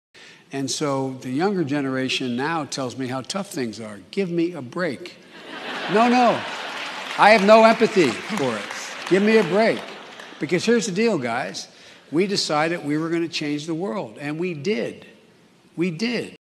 Joe Biden slams young people
"The younger generation now tells me how tough things are, give me a break ... I have no empathy,"  he says in this video.